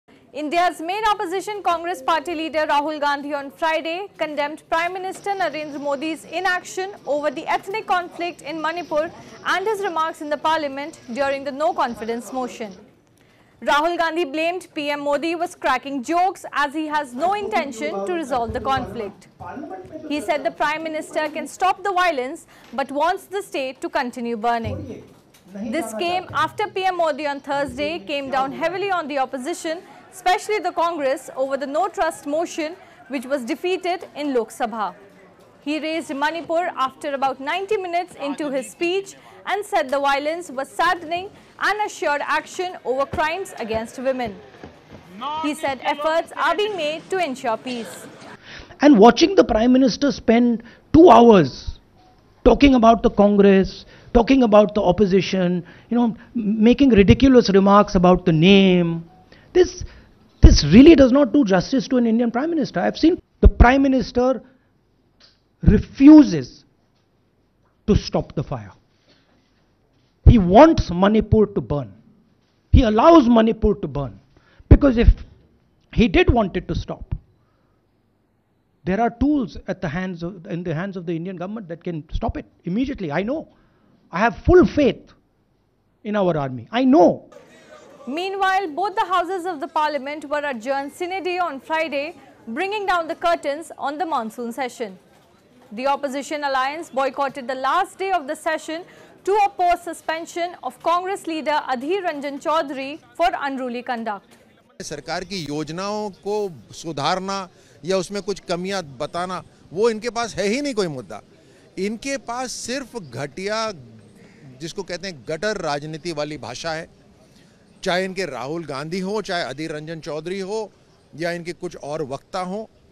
Rahul Gandhi condemns PM Modi over remarks in parliament, Monsoon Session concludes
Indian’s main Opposition Congress party leader Rahul Gandhi on Friday condemned Prime Minister Narendra Modi’s inaction over the ethnic conflict in Manipur and his remarks in the parliament during the no-confidence motion. Rahul Gandhi blamed PM Modi was cracking jokes as he has no intention to resolve the conflict. He said the Prime Minister can stop the violence, but wants the state to continue burning. This came after PM Modi on Thursday came down heavily on the Opposition, especially the Congress, over the no-trust motion, which was defeated in Lok Sabha. He raised Manipur after about 90 minutes into his speech, and said the violence was saddening and assured action over crimes against women. He said efforts are being made to ensure peace.